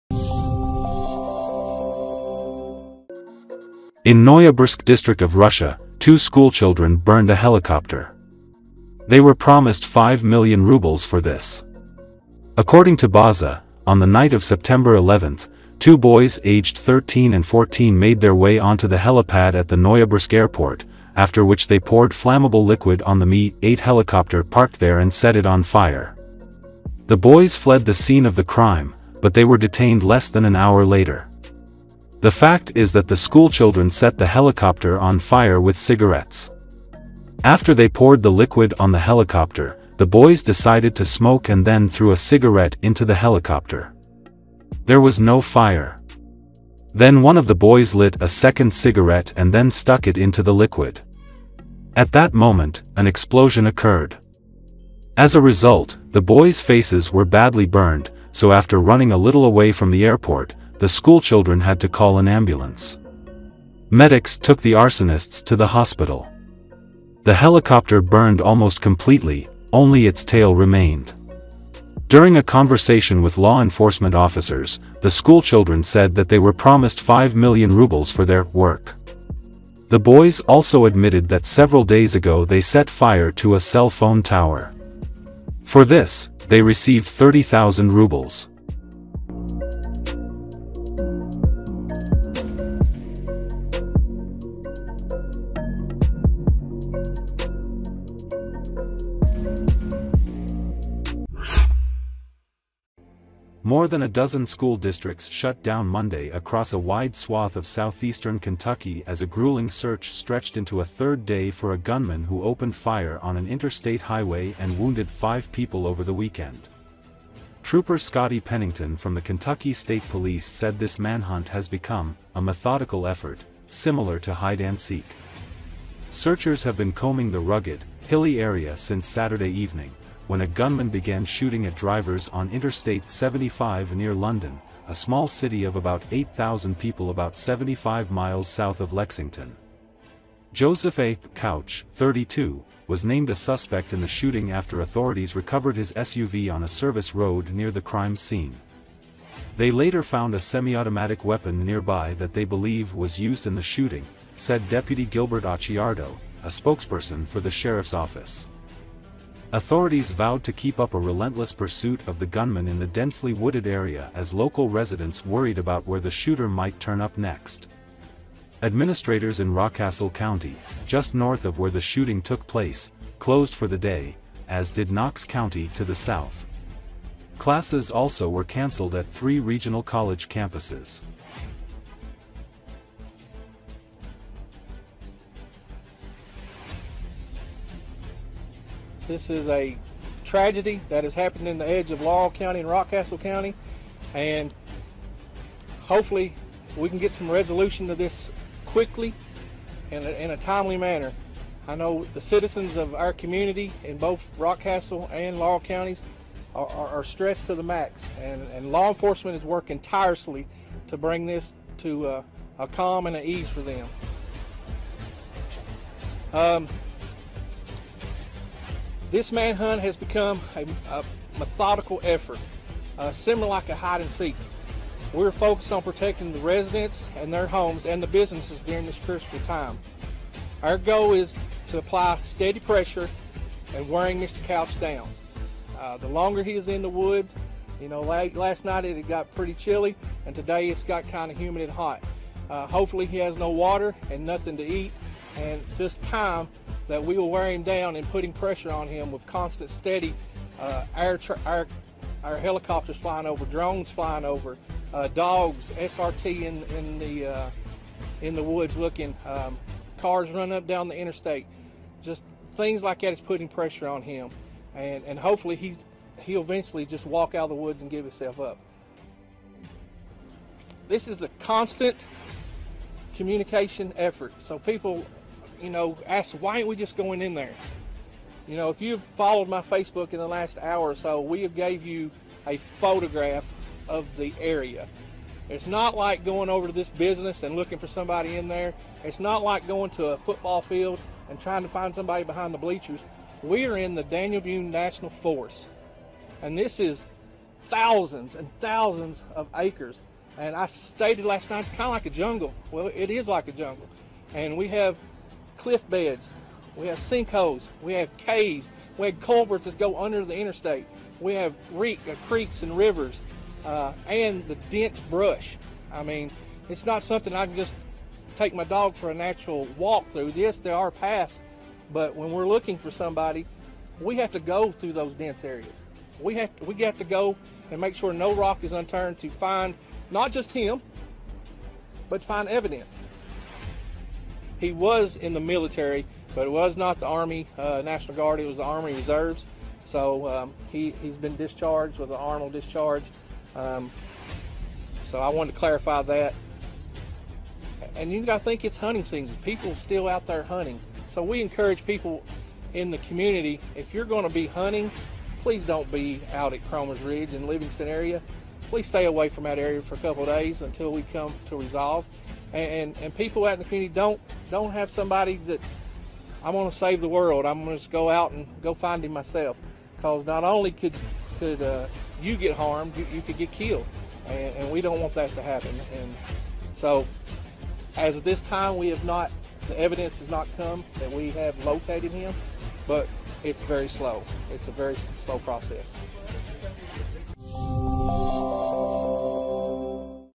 Two schoolchildren burned a Russian helicopter in Noyabrsk airport
In Noyabrsk district of Russia, two schoolchildren burned a helicopter. They were promised 5 million rubles for this.

According to Baza, on the night of September 11, two boys aged 13 and 14 made their way onto the helipad at the Noyabrsk airport, after which they poured flammable liquid on the Mi-8 helicopter parked there and set it on fire.

The boys fled the scene of the crime, but they were detained less than an hour later. The fact is that the schoolchildren set the helicopter on fire with cigarettes. After they poured the liquid on the helicopter, the boys decided to smoke and then threw a cigarette into the helicopter. There was no fire. Then one of the boys lit a second cigarette and then stuck it into the liquid. At that moment, an explosion occurred.

As a result, the boys' faces were badly burned, so after running a little away from the airport, the schoolchildren had to call an ambulance. Medics took the arsonists to the hospital. The helicopter burned almost completely - only its tail remained. During a conversation with law enforcement officers, the schoolchildren said that they were promised 5 million rubles for their "work". The boys also admitted that several days ago they set fire to a cell phone tower. For this, they received 30 thousand rubles.